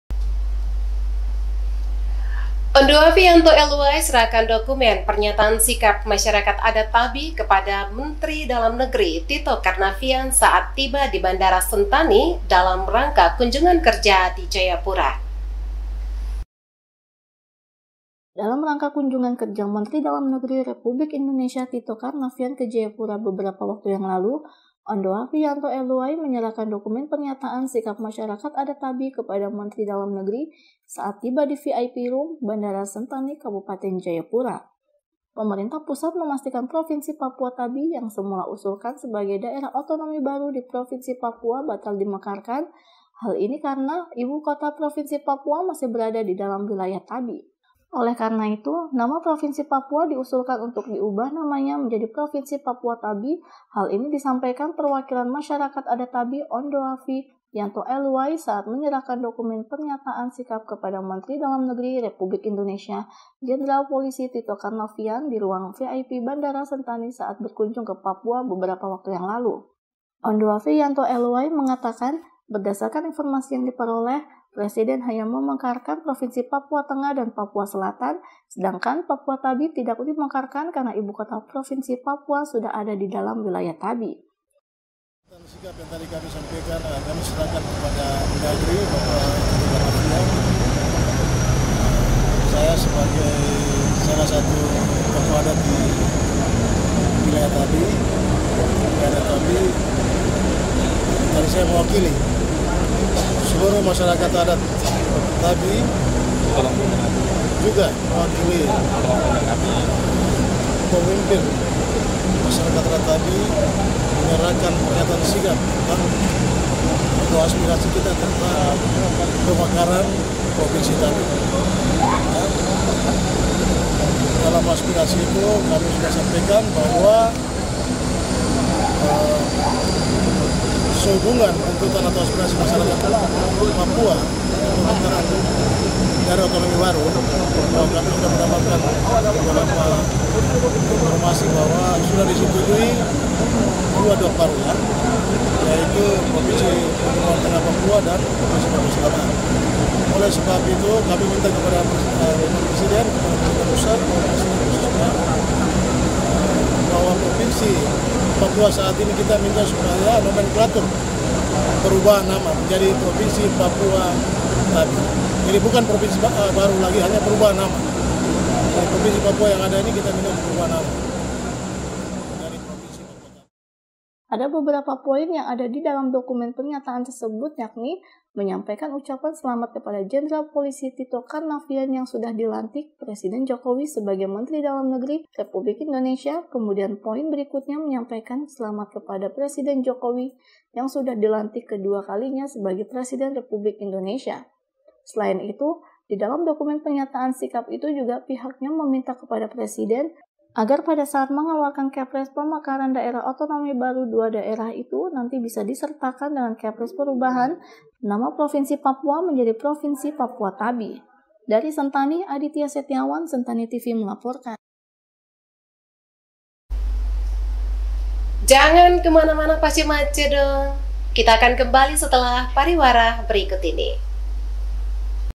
Yanto Eluay Serahkan Pernyataan Ke MENDAGRI | Sentani TV
BERIKUT HIGHLIGHT BERITA EDISI Selasa, 12 November  2019